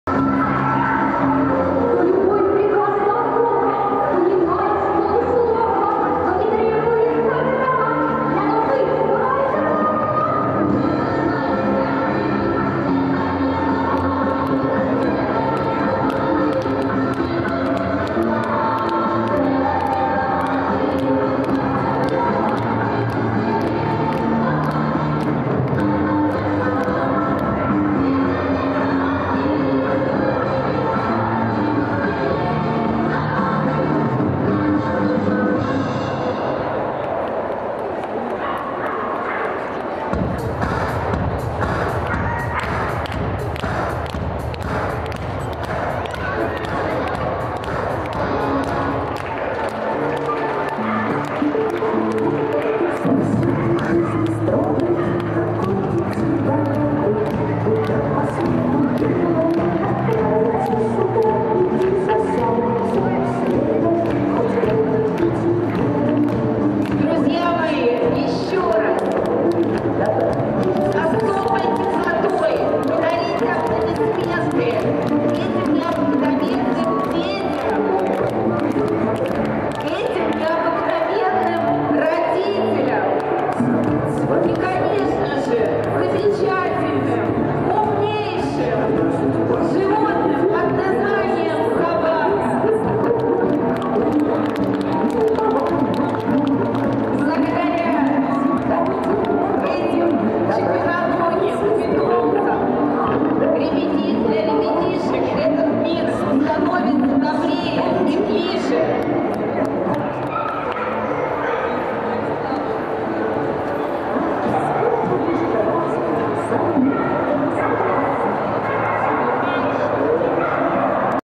Выставка собак всех пород CACIB 2013 в Новосибирске, Часть 8
Фоторепортажи (Новосибирский Фотоблог) CACIB 2013, 8 декабря 2013 года, Экспоцентр, Новосибирск